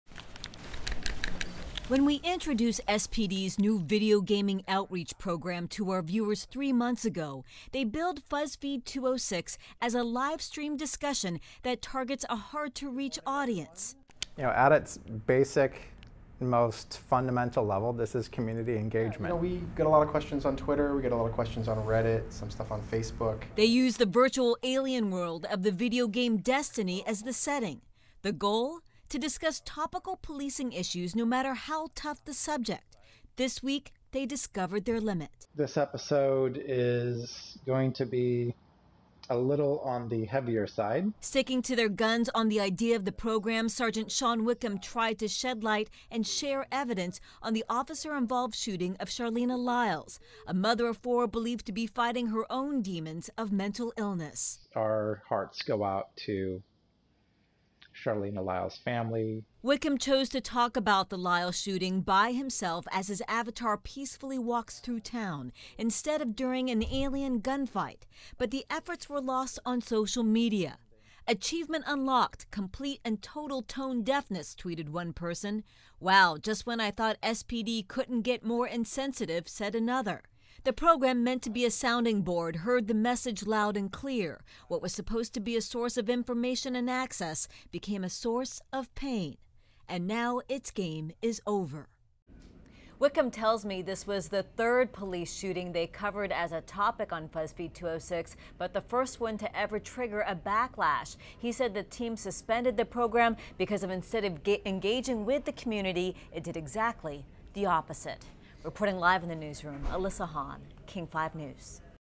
SPD cancels 'Fuzzfeed206' after backlash
Seattle police suspend a video game outreach program after backlash over an online discussion about the Charleena Lyles case.